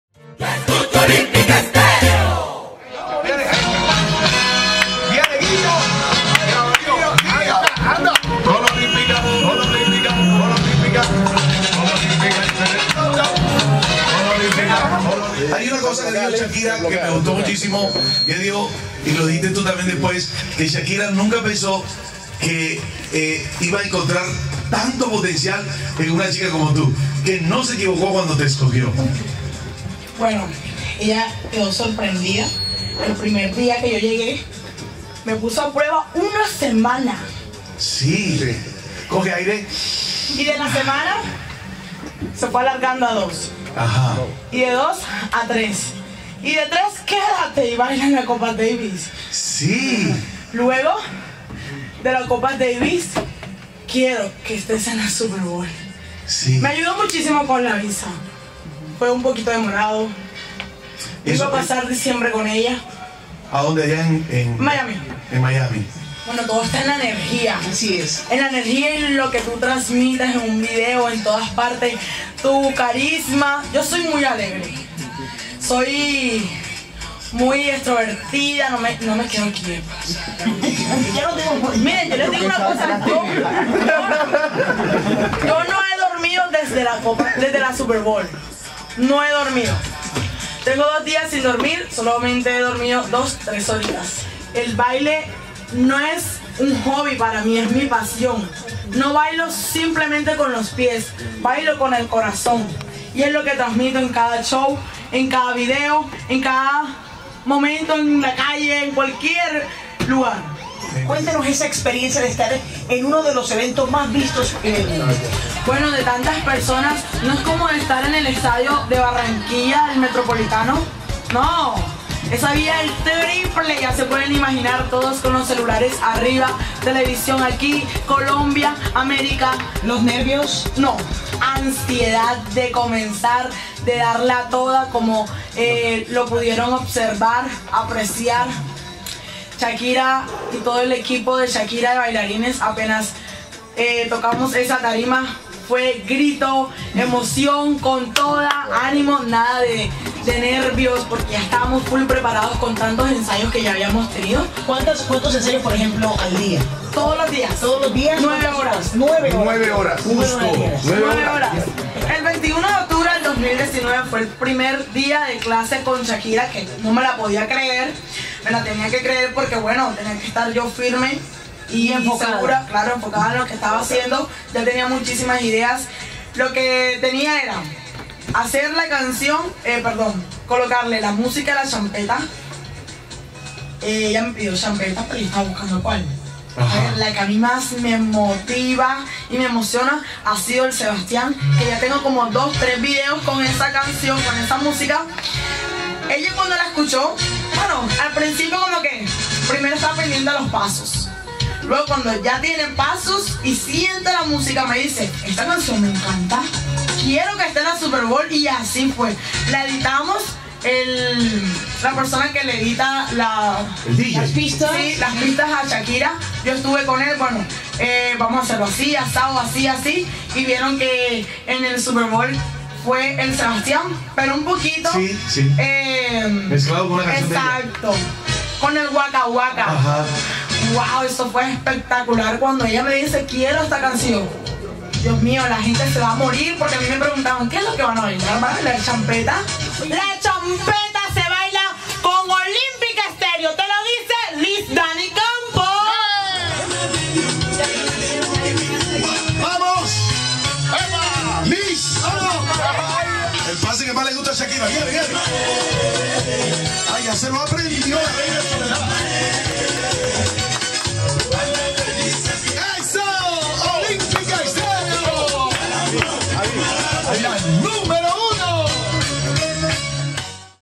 “Yo no bailo con los pies sino con el alma”, Liz Dany Campo Díaz, coreógrafa de Shakira
En entrevista con Olímpica Stereo, la responsable de la coreografía de la brillante presentación que tuvo Shakira en el Super Bowl, contó detalles de todo el proceso que vivió junto a la cantante.